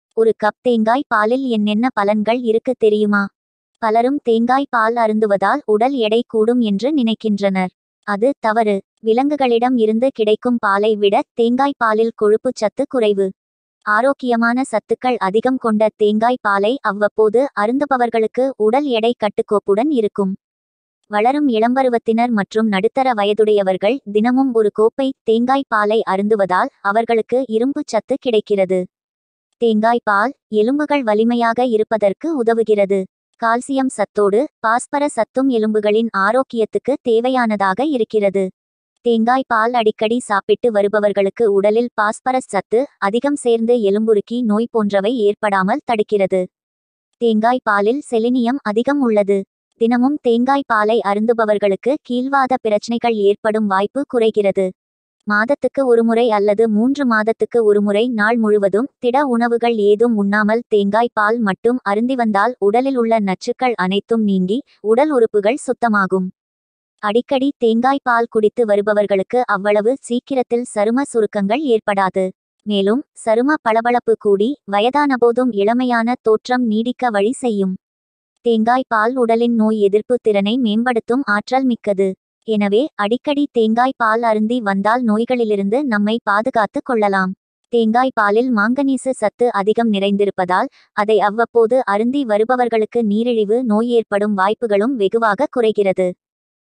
ஒரு கப் தேங்காய் பாலில் என்னென்ன பலன்கள் இருக்கு தெரியுமா?
பலரும் தேங்காய் பால் அருந்துவதால் உடல் எடை கூடும் என்று நினைக்கின்றனர். அது தவறு. விலங்குகளிடம் இருந்து கிடைக்கும் பாலை விட தேங்காய் பாலில் கொழுப்புச் சத்து குறைவு.

ஆரோக்கியமான சத்துக்கள் அதிகம் கொண்ட தேங்காய் பாலை அவ்வப்போது அருந்துபவர்களுக்கு உடல் எடை கட்டுக்கோப்புடன் இருக்கும்.

வளரும் இளம்பருவத்தினர் மற்றும் நடுத்தர வயதுடையவர்கள் தினமும் ஒரு கோப்பை தேங்காய் பாலை அருந்துவதால் அவர்களுக்கு இரும்புச் சத்து கிடைக்கிறது.

தேங்காய் பால், எலும்புகள் வலிமையாக இருப்பதற்கு உதவுகிறது. கால்சியம் சத்தோடு, பாஸ்பர சத்தும் எலும்புகளின் ஆரோக்கியத்துக்கு தேவையானதாக இருக்கிறது.

தேங்காய் பால் அடிக்கடி சாப்பிட்டு வருபவர்களுக்கு உடலில் பாஸ்பரஸ் சத்து அதிகம் சேர்ந்து எலும்புருக்கி நோய் போன்றவை ஏற்படாமல் தடுக்கிறது.

தேங்காய் பாலில் செலினியம் அதிகம் உள்ளது. தினமும் தேங்காய் பாலை அருந்துபவர்களுக்கு கீல்வாத பிரச்னைகள் ஏற்படும் வாய்ப்பு குறைகிறது.

மாதத்துக்கு ஒருமுறை அல்லது மூன்று மாதத்துக்கு ஒருமுறை நாள் முழுவதும் திட உணவுகள் ஏதும் உண்ணாமல் தேங்காய் பால் மட்டும் அருந்திவந்தால் உடலில் உள்ள நச்சுக்கள் அனைத்தும் நீங்கி, உடல் உறுப்புகள் சுத்தமாகும்.

அடிக்கடி தேங்காய் பால் குடித்து வருபவர்களுக்கு அவ்வளவு சீக்கிரத்தில் சரும சுருக்கங்கள் ஏற்படாது. மேலும், சரும பளபளப்பு கூடி, வயதானபோதும் இளமையான தோற்றம் நீடிக்க வழி செய்யும்.

தேங்காய் பால் உடலின் நோய் எதிர்ப்புத் திறனை மேம்படுத்தும் ஆற்றல் மிக்கது. எனவே, அடிக்கடி தேங்காய் பால் அருந்தி வந்தால் நோய்களிலிருந்து நம்மை பாதுகாத்துக் கொள்ளலாம்.

தேங்காய் பாலில் மாங்கனீசு சத்து அதிகம் நிறைந்திருப்பதால், அதை அவ்வப்போது அருந்தி வருபவர்களுக்கு நீரிழிவு நோய் ஏற்படும் வாய்ப்புகளும் வெகுவாகக் குறைகிறது.